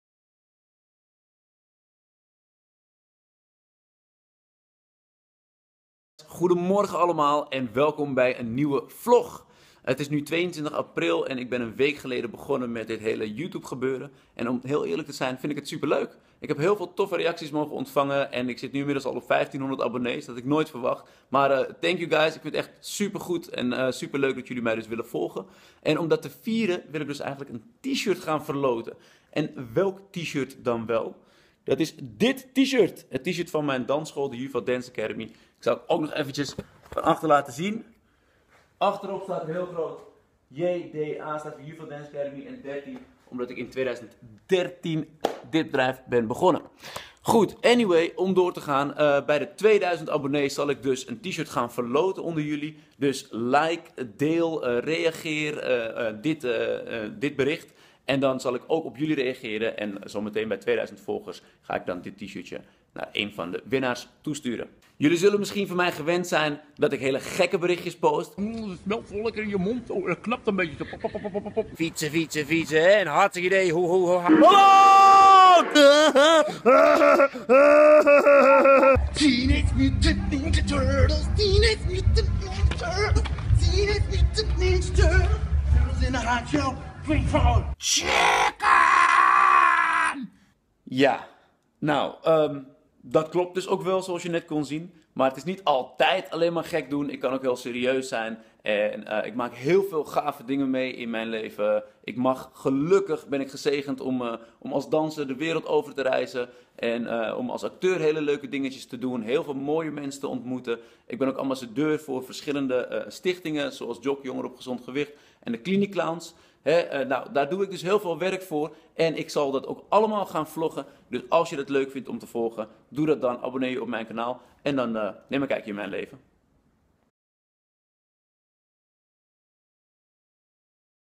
WINACTIE | T-shirt | Juvat Westendorp
Ik ben heel erg dankbaar voor het aantal abonnees dat ik nu al heb en om dat te vieren verloot ik bij 2000 abonnees een T-Shirt (in maat naar keuze) onder mijn volgers.

Vinden jullie de dingen die ik doe leuk?
Like, reageer op, en deel dan mijn video's en wellicht komt het JDA T-shirt dan gauw jouw kant op.